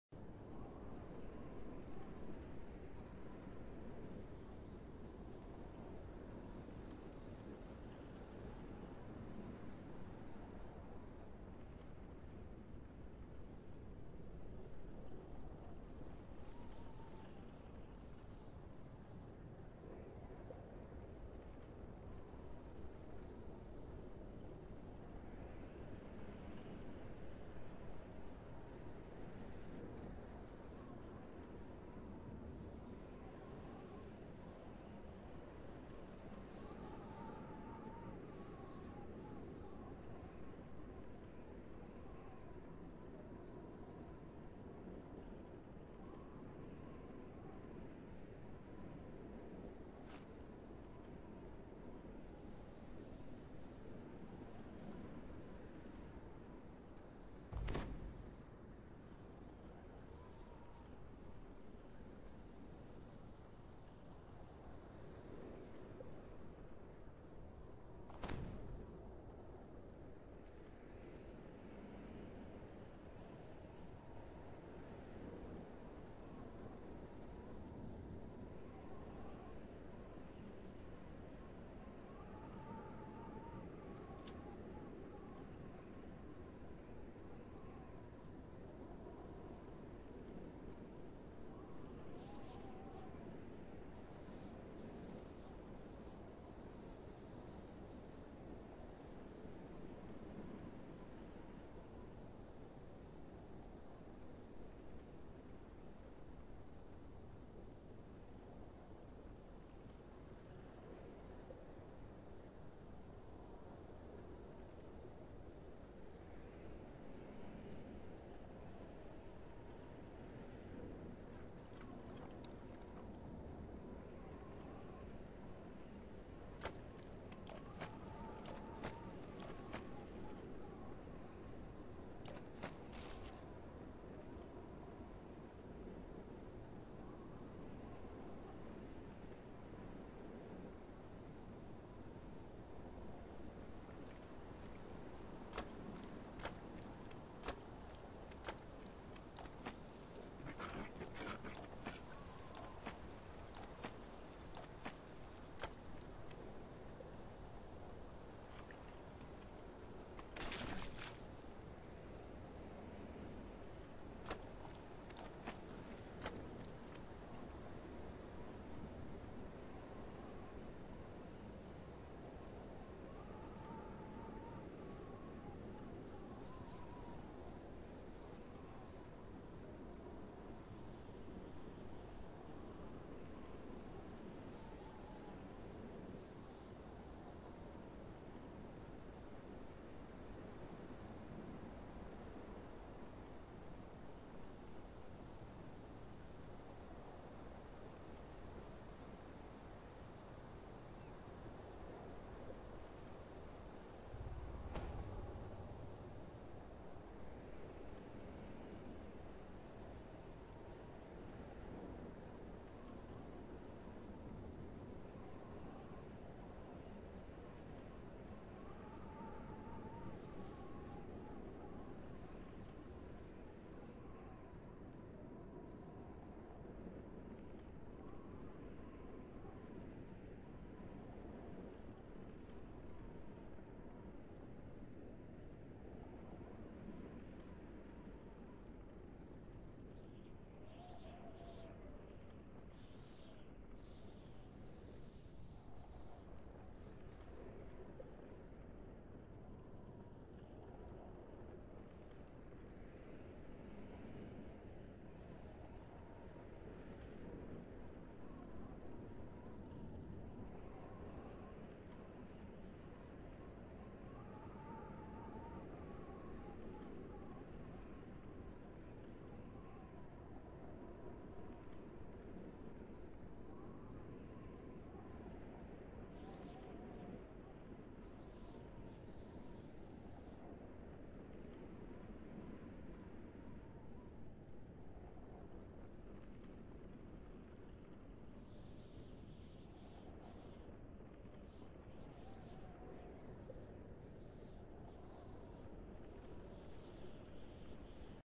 Otto's Box with stone of XP.
Otto's Box with stone of xp. Just a quick show of what is in the box =)